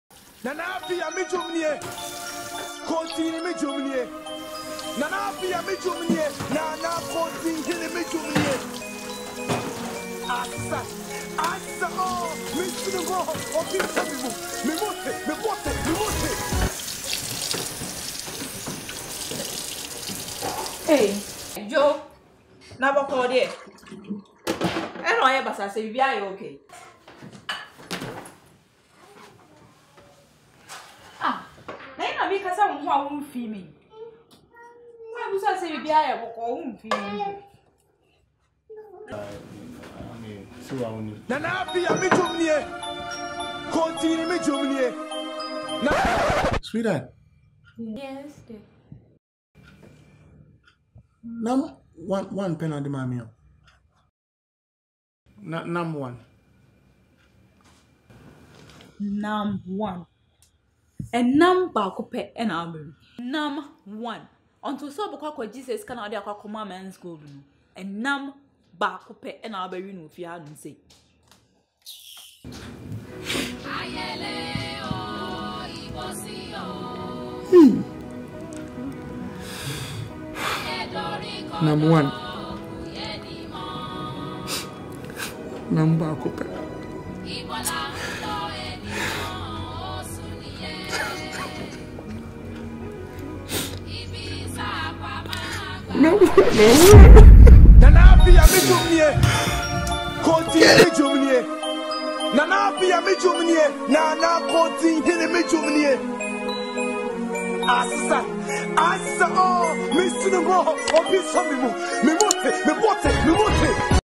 NAM1-Kwabena & Ama Nonsense Series (Am TV Columbus)
kwabena becomes a victim  of NAM1 saga in his pocket and in the kitchen as well